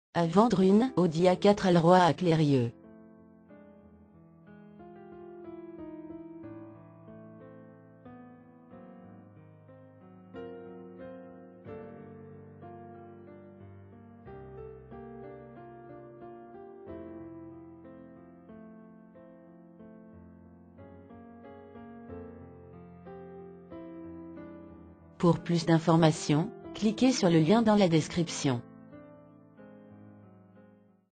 Achat Vente une AUDI A4 ALLROAD Clérieux
A vendre à  Clérieux  Occasion automobile AUDI A4 ALLROAD de 68500 Km.
automobile occasion Autoenvideo : Achat Vente une AUDI A4 ALLROAD  Clérieux  Drôme